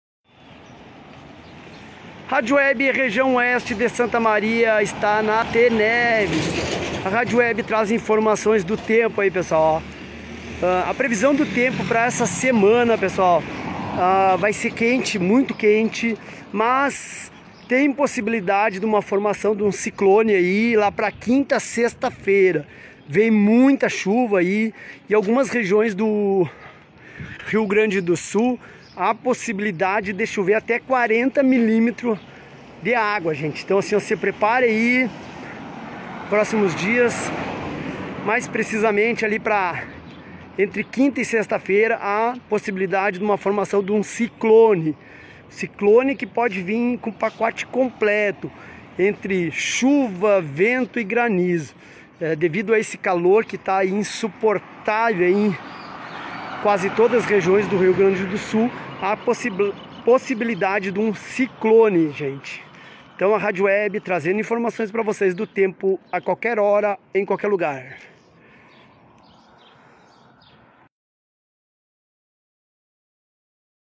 22 de novembro de 2021
RADIO WEB REGIAO OESTE S:A formação de um ciclone vai mudar o tempo no Rio Grande do Sul, deixando Estado em alerta para temporais.